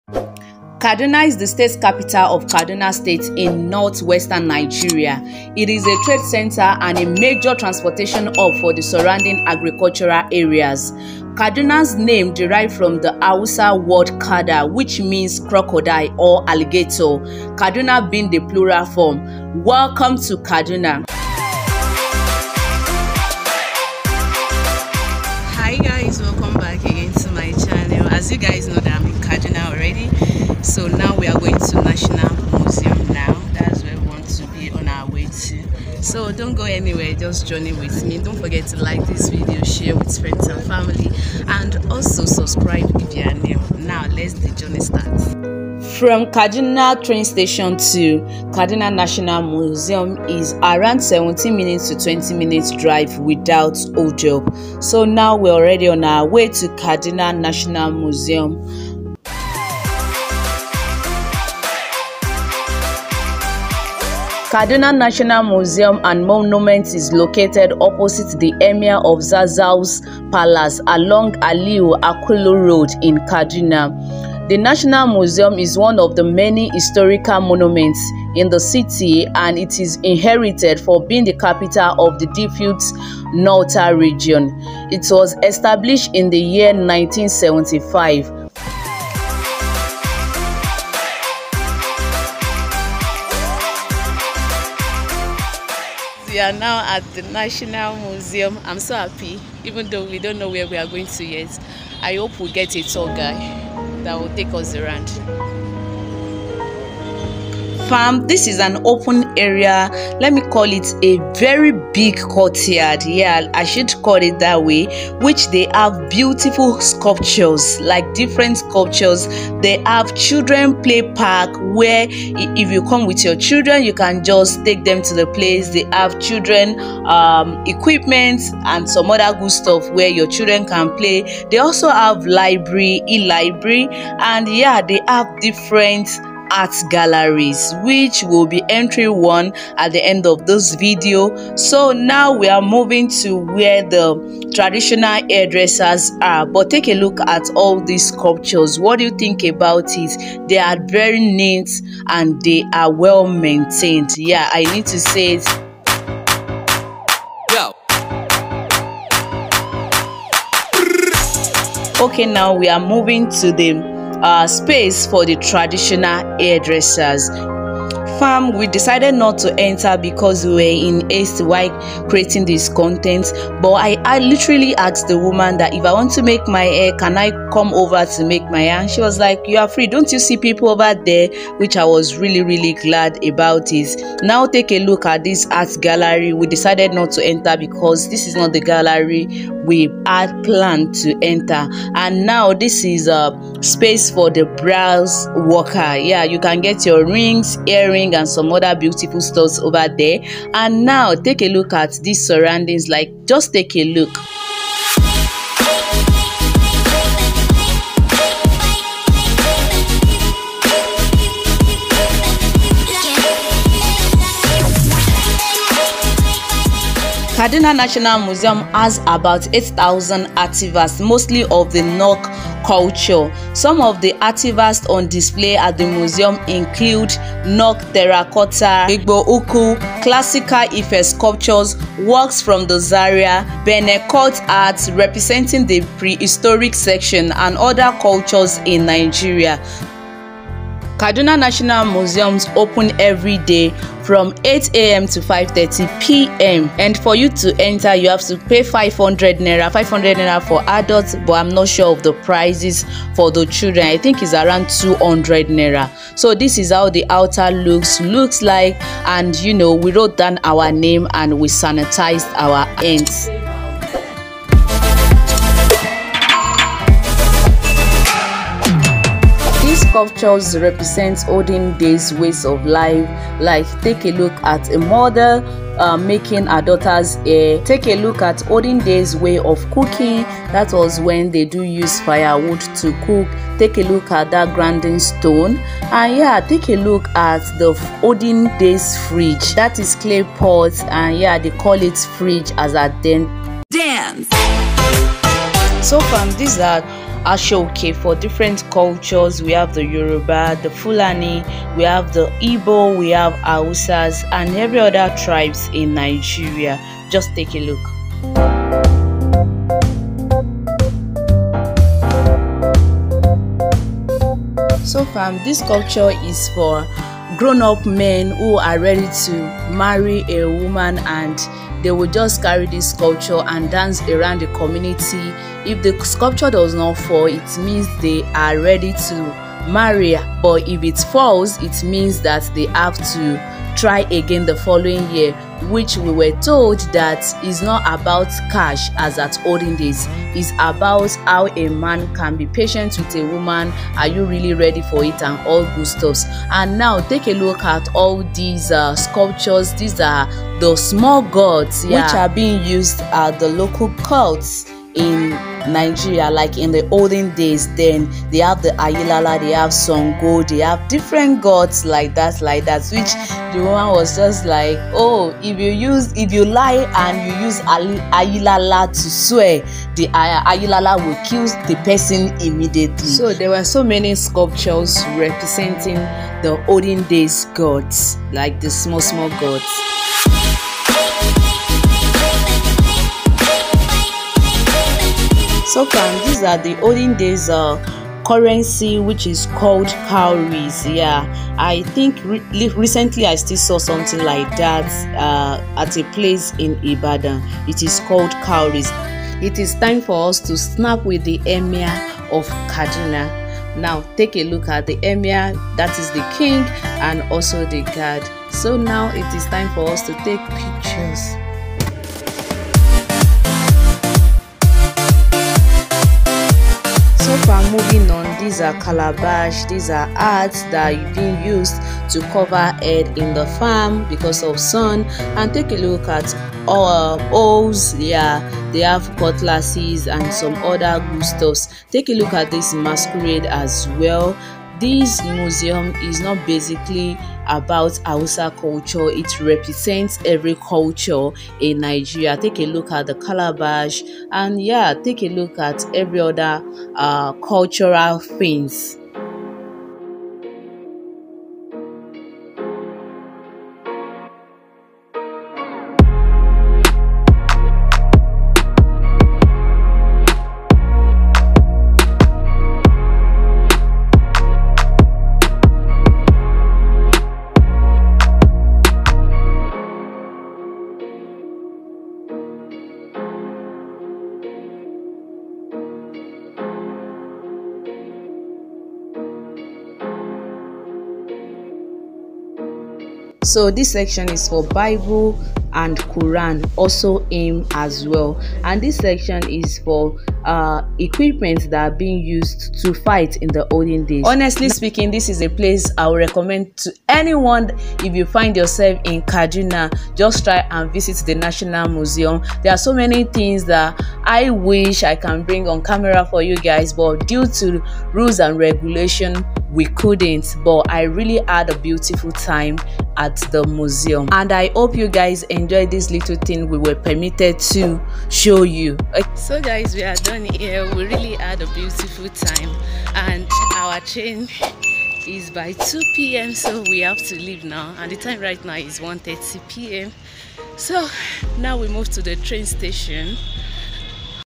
A trip to Kaduna national museum where all cultures are well represented + meet Emir of Kaduna
Thanks for stopping by to watch this video I really do appreciate

Your suggestion is always welcome.